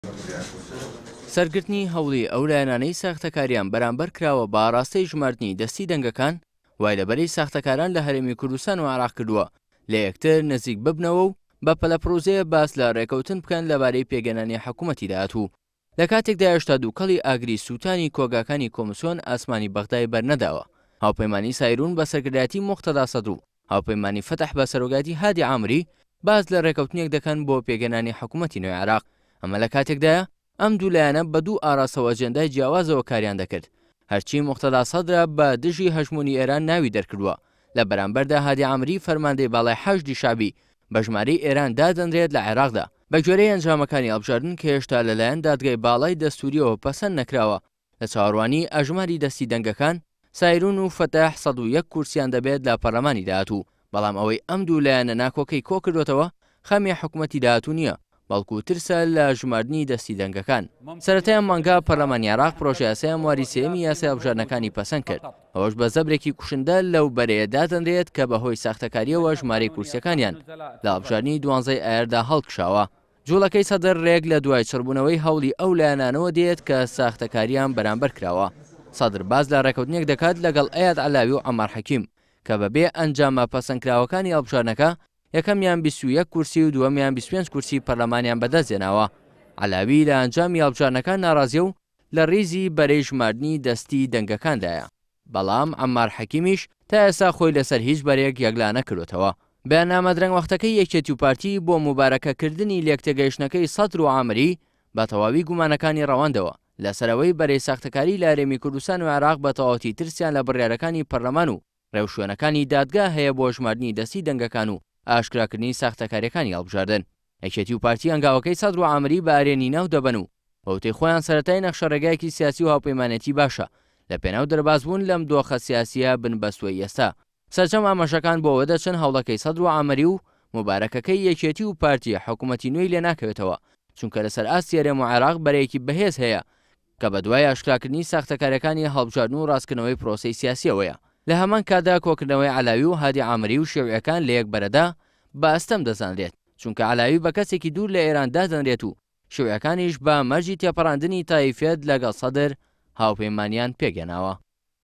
جموجوڵێكی دیكەی بەرەی ساختەكاران
سەدر‌و عامری ‌و یەكێتی ‌و پارتی ترسیان لە ئەژماری دەستی هەیە
سەدر لە ترسی ژماردنی دەستی دەنگەكان لە هاوپەیمانێكی ئێران نزیك دەبێتەوە
یەكێتی ‌و پارتی لە ترسی ژماردنی دەستی دەنگەكان خۆیان لە سەدر ‌و عامری نزیك دەكەنەوە

لە كاتێكدا هەموو ئاماژەكان بە ئاڕاستەی ژماردنی دەستی دەنگەكان دەچن، موقتەدا سەدر ‌و هادی عامری كە یەكێكیان لەگەڵ ئێران ‌و ئەویتریشیان دژیەتی لەگەڵ یەكێتی ‌و پارتی باس لە رێككەوتن دەكەن بۆ پێكهێنانی حكومەتی نوێ.

كۆبونەوەی ئەم لایەنە نەیارانە هاوكاتە لەگەڵ هەوڵی لایەنەكان بۆ ژماردنی دەستی دەنگەكان، كە ئەوە دەردەخات ئەم لایەنانە ساختەكاریی گەورەیان لە هەڵبژاردنەكەی 12 ئایاردا كردو‌وە.

سەرگرتنی هەوڵی ئەو لایەنانەی ساختەكارییان بەرامبەر كراوە بە ئاراستەی ژماردنی دەستی دەنگەكان، وای لەبەرەی ساختەكاران لە هەرێمی كوردستان ‌و عێراق كردو‌وە، لە یەكتر نزیك ببنەوە‌و بە پەلە پڕوزێ‌ باس لە رێككەوتن بكەن لەبارەی پێكهێنانی حكومەتی داهاتوو.

لە كاتێكدا هێشتا دو‌وكەڵی ئاگری سوتانی كۆگاكانی كۆمسیۆن ئاسمانی بەغدای بەرنەداوە، هاوپەیمانی سائیرون بەسەركردایەتی موقتەدا سەدر ‌و هاوپەیمانی فەتح بەسەرۆكایەتی هادی عامری باس لە رێككەوتنێك دەكەن بۆ پێكهێنانی حكومەتی نوێی عێراق.

ئەمە لە كاتێكدایە ئەم دوو لایەنە بە دوو ئاڕاستە ‌و ئەجێندای جیاوازەوە كاریان دەكرد، هەرچی موقتەدا سەدرە بە دژی هەژمونی ئێران ناوی دەركرد‌وە، لە بەرامبەردا هادی عامری، فەرماندەی باڵای حەشدی شەعبی بە ژمارەی ئێران دادەنرێت لە عێراقدا.

بەگوێرەی ئەنجامەكانی هەڵبژاردن كە هێشتا لەلایەن دادگای باڵای دەستورییەوە پەسەند نەكراوە لە چاوەڕوانی ئەژماریی دەستی دەنگەكان، سائیرون ‌و فەتح 101 كورسیان دەبێت لە پەرلەمانی داهاتوو، بەڵام ئەوەی ئەم دوو لایەنە ناكۆكەی كۆكرد‌ووەتەوە، خەمی حكومەتی داهاتوو نیە، بەڵكو ترسە لە ژماردنی دەستی دەنگەكان.

سەرەتای ئەم مانگە، پەرلەمانی عێراق پرۆژە یاسای هەمواری سێیەمی یاسای هەڵبژاردنەكانی پەسەند كرد، ئەوەش بە زەبرێكی كوشندە لەو بەرەیە دادەنرێت كە بەهۆی ساختەكارییەوە ژمارەی كورسیەكانیان لە هەڵبژاردنی 12ی ئایاردا هەڵكشاوە.

جوڵەكەی سەدر رێك لە دوای چڕبو‌ونەوەی هەوڵی ئەو لایەنانەوە دێت كە ساختەكارییان بەرامبەر كراوە.

سەدر باس لە رێككەوتنێك دەكات لەگەڵ ئەیاد عەلاوی ‌و عەممار حەكیم، كە بەبێ‌ ئەنجامە پەسەند نەكراوەكانی هەڵبژاردنەكە، یەكەمیان 21 كورسی ‌و دووەمیان 25 كورسی پەرلەمانیان بە دەستهێناوە. عەلاوی لە ئەنجامی هەڵبژاردنەكان ناڕازییە ‌و لە ریزی بەرەی ژماردنی دەستی دەنگەكاندایە، بەڵام عەممار حەكیمیش تا ئێستا خۆی لەسەر هیچ بەرەیەك یەكلا نەكرد‌ووەتەوە.

بەیاننامە درەنگ وەختەكەی یەكێتی ‌و پارتی بۆ موبارەكەكردنی لێك تێگەیشتنەكەی سەدر‌و عامری، بە تەواوەتی گومانەكانی رەواندەوە، لەسەر ئەوەی بەرەی ساختەكاریی لە هەرێمی كوردستان ‌و عێراق بە تەواوەتی ترسیان لە بڕیارەكانی پەرلەمان ‌و رێوشوێنەكانی دادگا هەیە بۆ ژماردنی دەستی دەنگەكان ‌و ئاشكراكردنی ساختەكارییەكانی هەڵبژاردن.

 یەكێتی ‌و پارتی، هەنگاوەكەی سەدر ‌و عامری بە ئەرێنی ناودەبەن ‌و بە وتەی خۆیان سەرەتای نەخشە رێگایەكی سیاسی ‌و هاوپەیمانێتی باشە، لە پێناو دەربازبوون لەم دۆخە سیاسسییە بن بەستووەی ئێستا.

سەرجەم ئاماژەكان بۆ ئەوە دەچن هەوڵەكەی سەدر‌و عامری ‌و موبارەكەكەی یەكێتی ‌و پارتی حكومەتی نوێی لێ ناكەوێتەوە، چونكە لەسەر ئاستی هەرێم ‌و عێراق، بەرەیەكی بەهێز هەیە كە بەدوای ئاشكراكردنی ساختەكارییەكانی هەڵبژاردن ‌و راستكردنەوەی پرۆسەی سیاسیی هەیە.

لە هەمان كاتدا كۆكردنەوەی عەلاوی ‌و هادی عامری ‌و شیوعییەكان لەیەك بەرەدا بە ئەستەم دەزانرێت، چونكە عەلاوی بەكەسێكی دور لە ئێران دادەنرێت ‌و شیوعییەكانیش بە مەرجی تێپەڕاندنی تائیفیەت لەگەڵ سەدر هاوپەیمانیان پێكهێناوە.